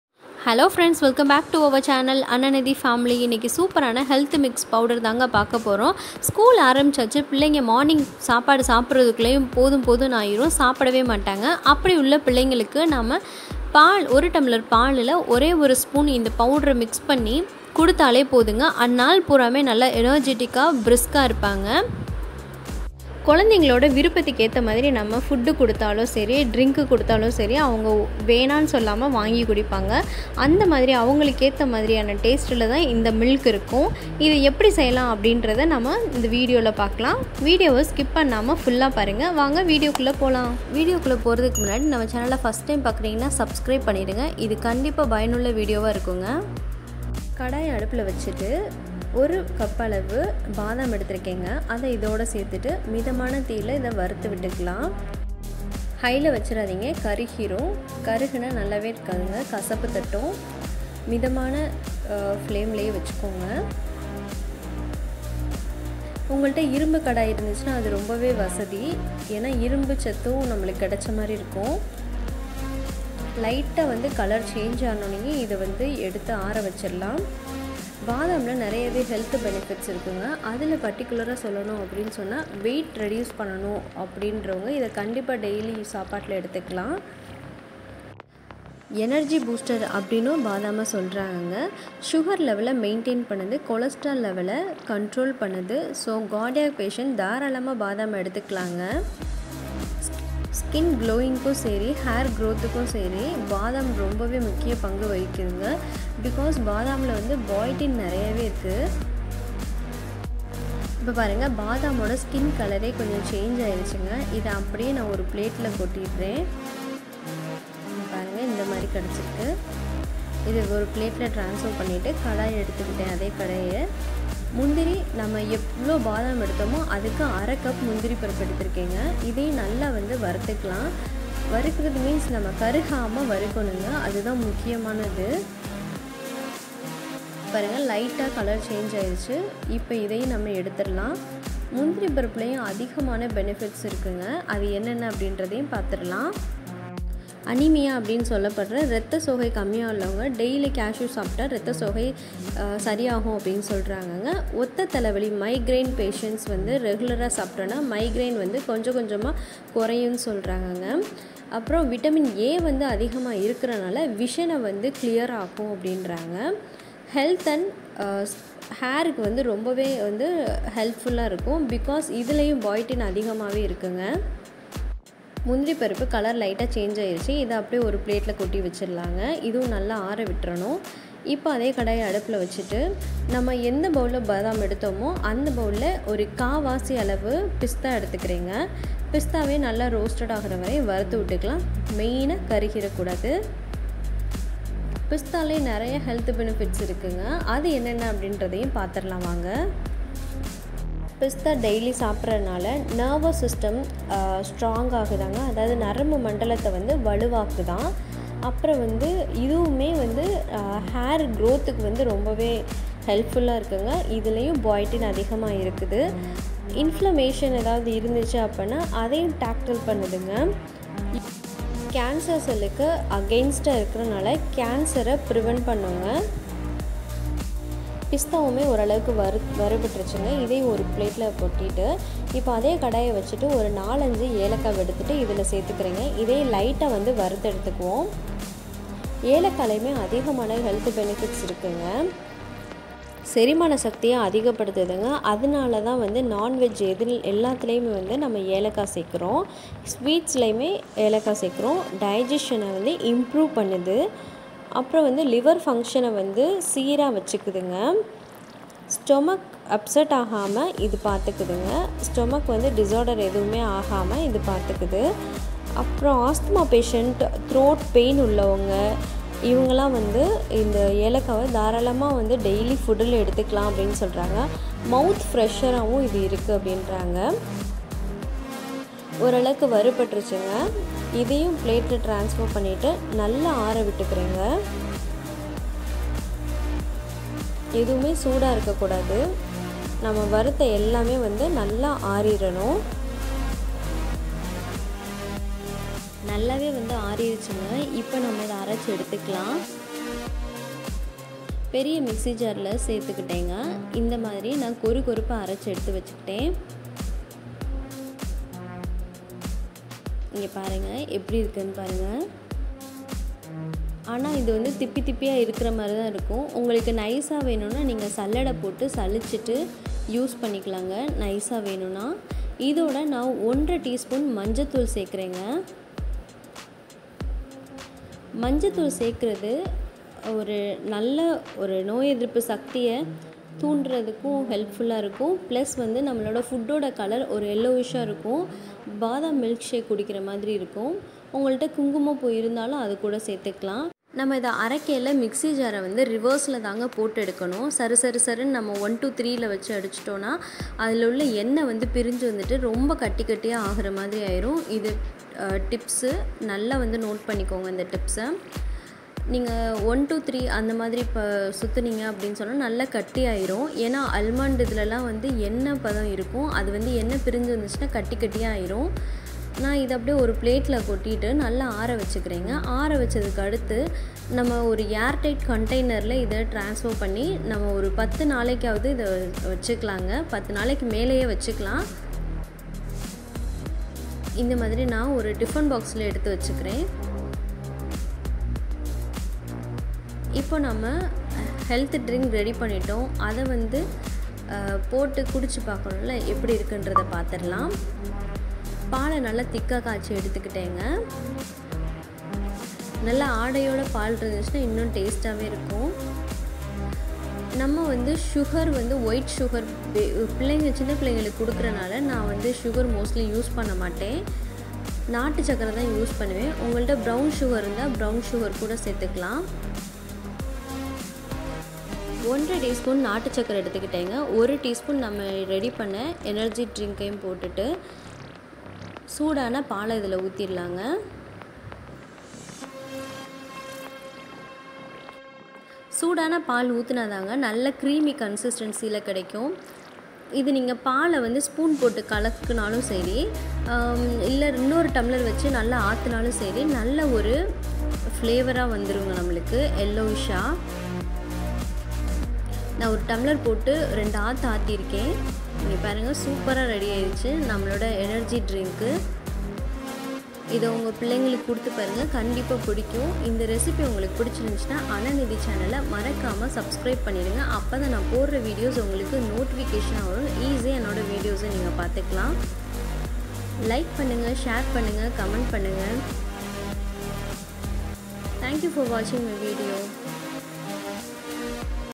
Brain Booster drink/Energy drink for kids and all/Health drink recipe in AnanAthi family Tamil
In this video we will see about How to make Brain Booster Nuts milk powder recipe in AnanAthi family Tamil..